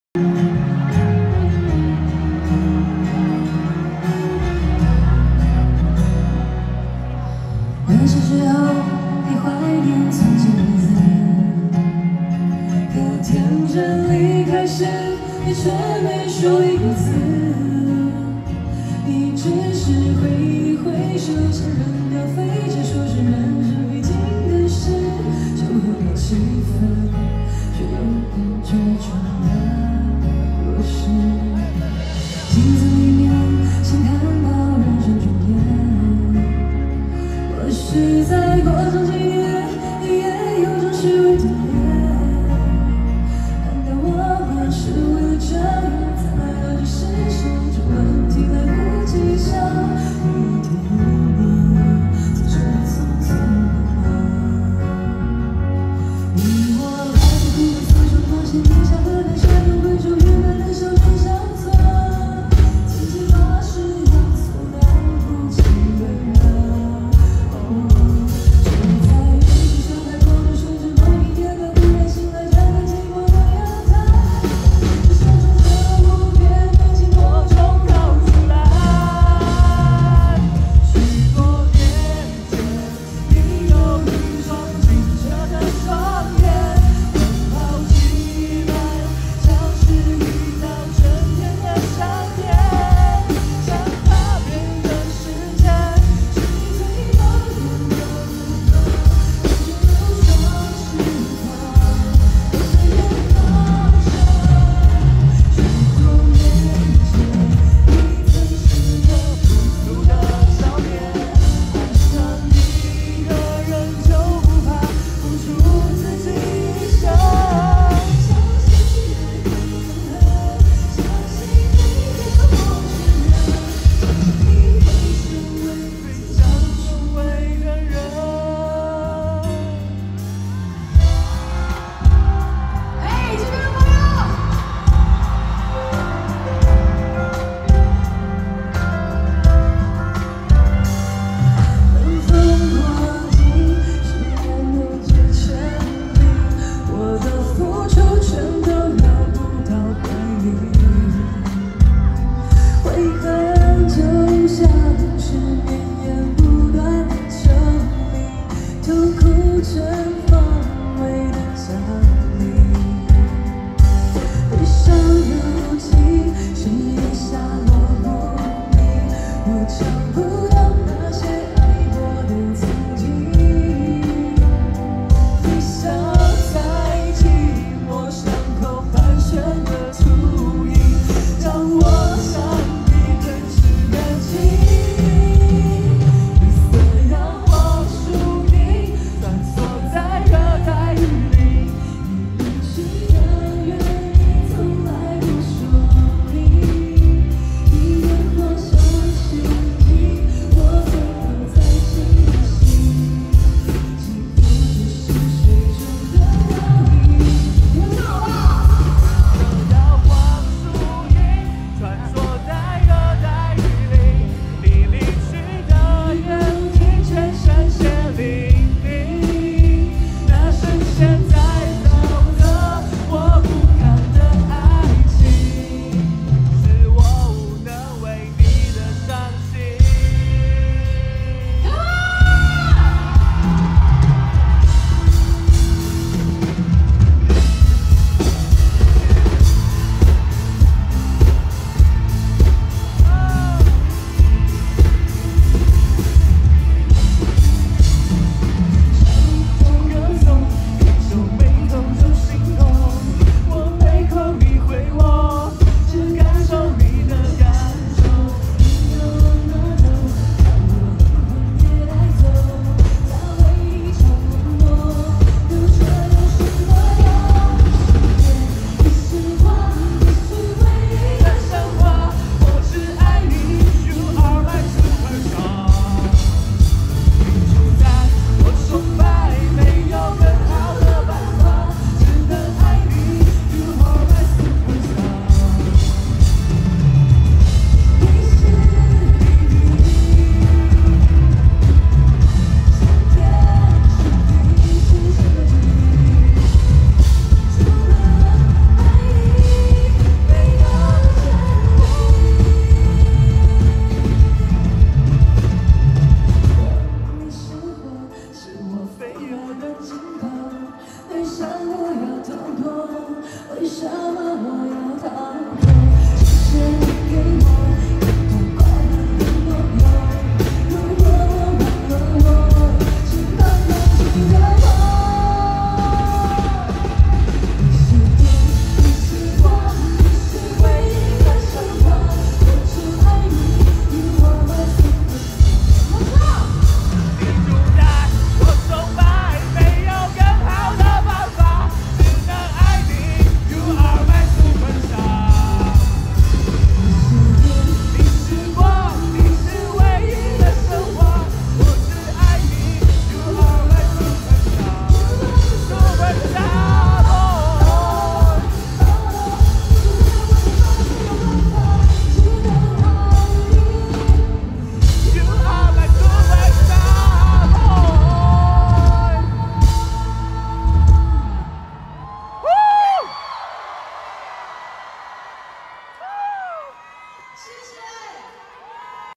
2024.01.20 Ella陳嘉樺 星曜萬州新年演唱會 天團組曲
影片轉自微博 @重慶文娛資訊